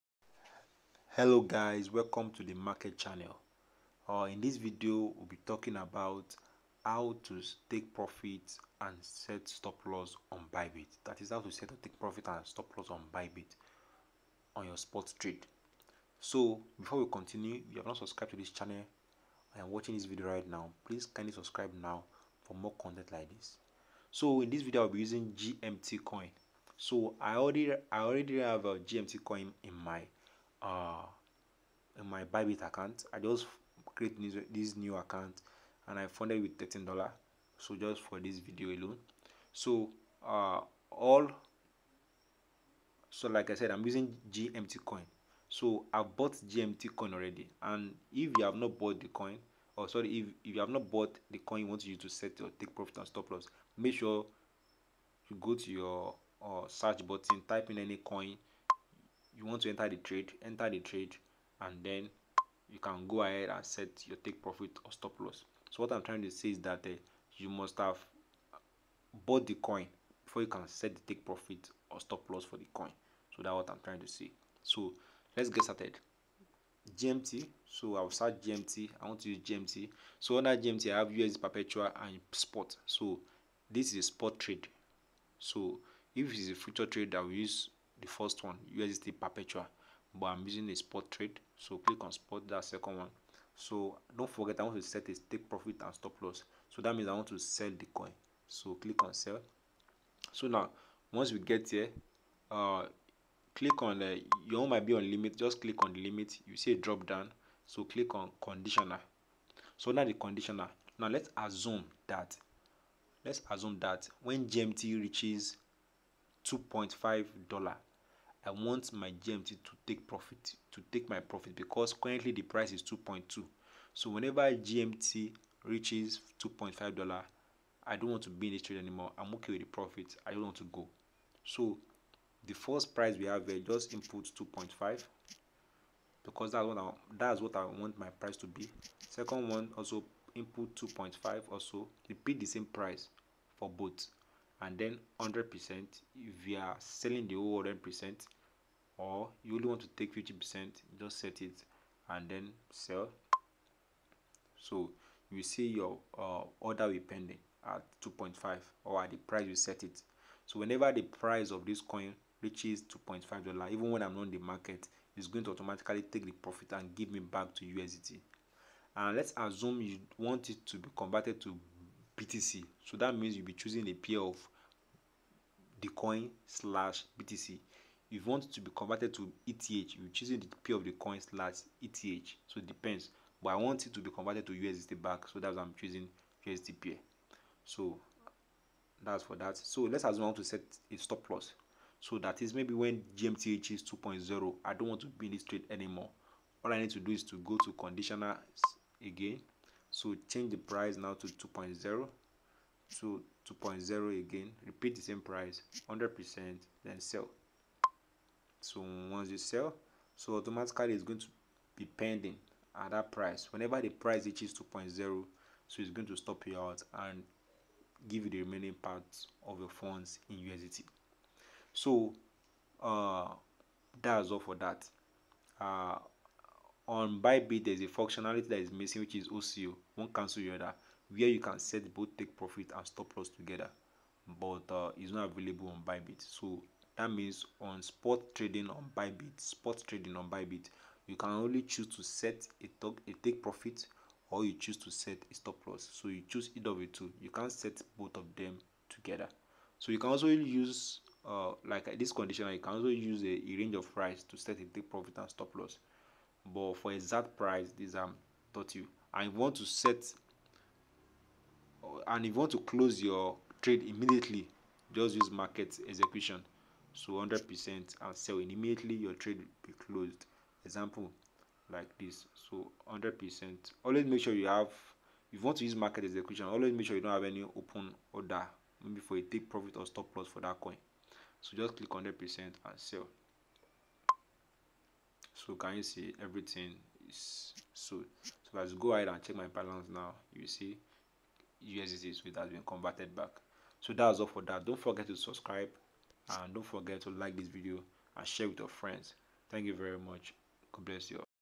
How to Set Stop Loss and Take Profit on Bybit Exchange
How to set Take Profit and Stop Loss on Bybit Exchange